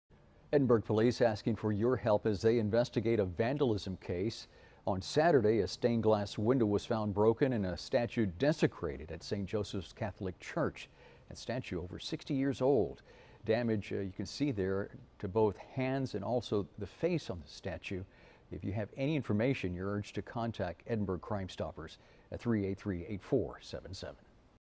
Police Investigating Vandalism Case at Edinburg Church
A stained glass window was found broken and a statue desecrated.